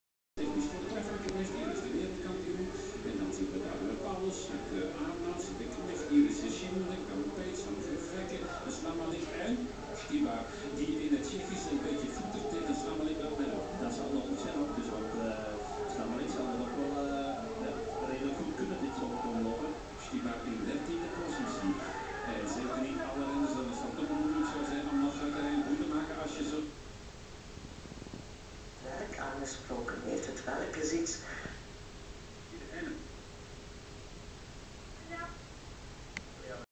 Biebel graven in de sneeuw.MOV
Graven in de sneeuw zoeken wat eronder zit